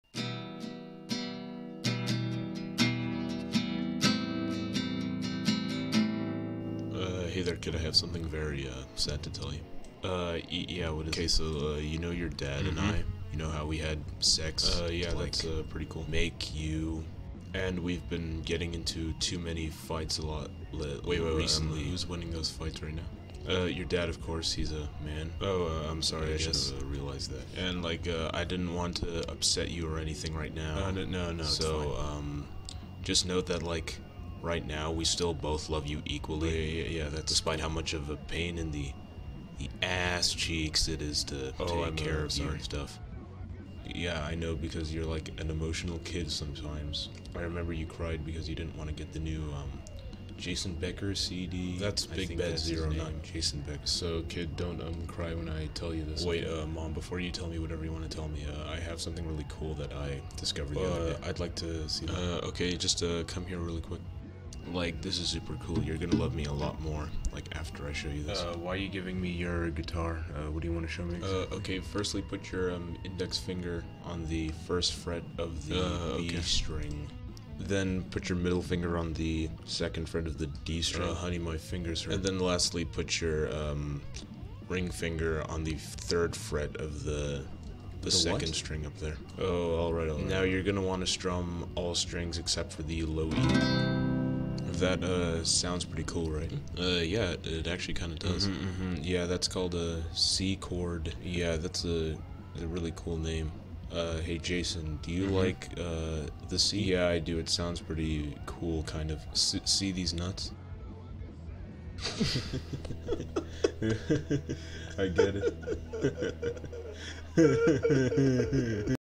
Parents have divorce and tell their son
Saddest thing Ive animated ngl